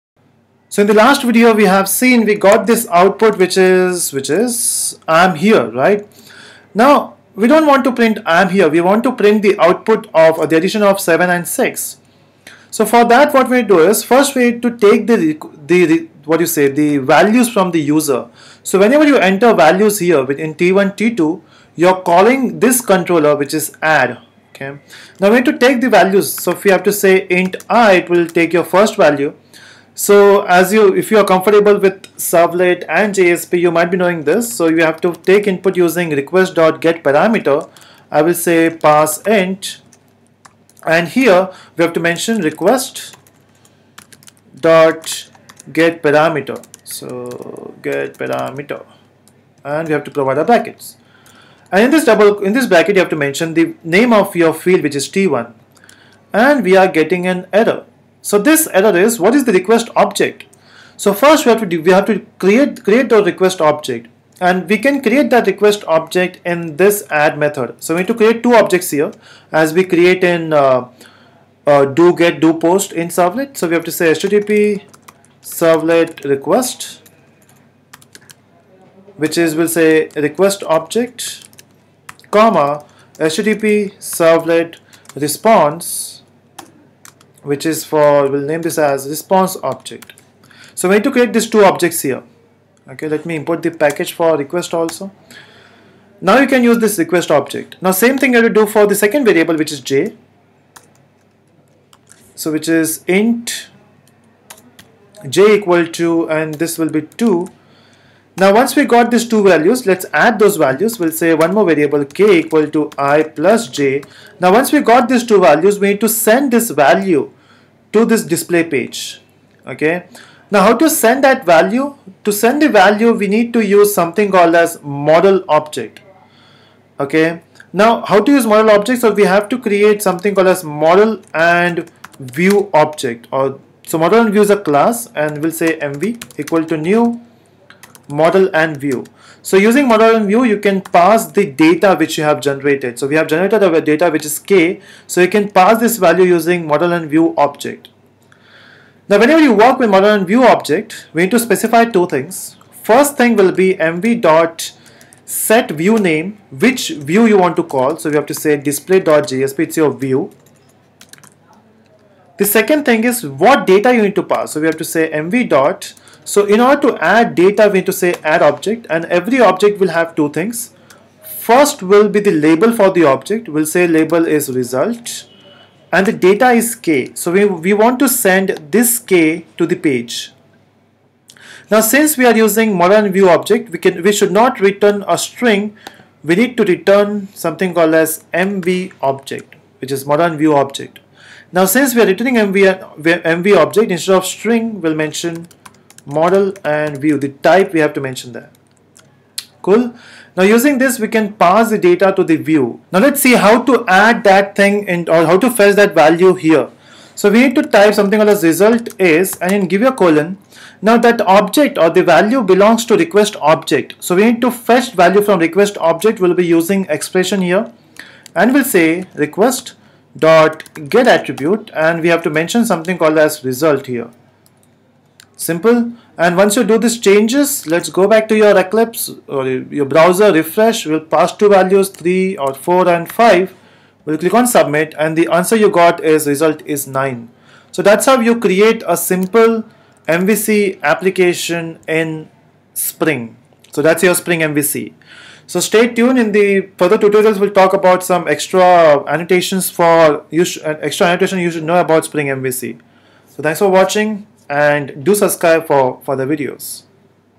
Spring MVC Tutorial with Example part 4
Adding two numbers in spring mvc:
@RequestMapping("/add")
how to add two numbers and dispaly